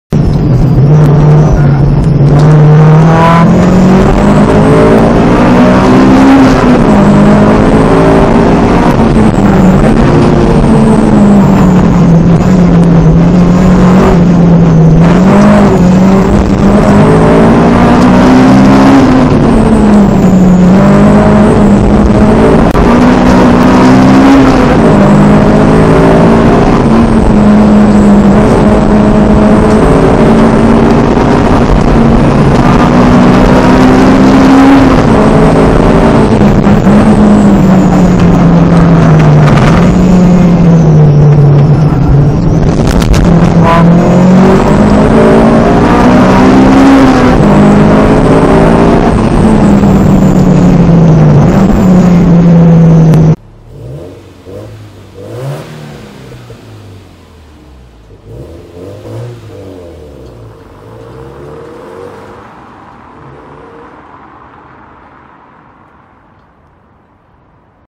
S2000 日光サーキット 40.557 2020/11/02 六連星
先日僕と一緒に日光サーキット走行会に参加した
かんくんのベストラップの動画です

仕様
AP2 2,2L
HKS Super Sound Master
HKS スポーツ触媒
無限 エキマニ
J'S RACING ECU
TE37SL 10J＋40
BS 71R 265/35/18
HKS ハイパーマックスSP
ボディ補強諸々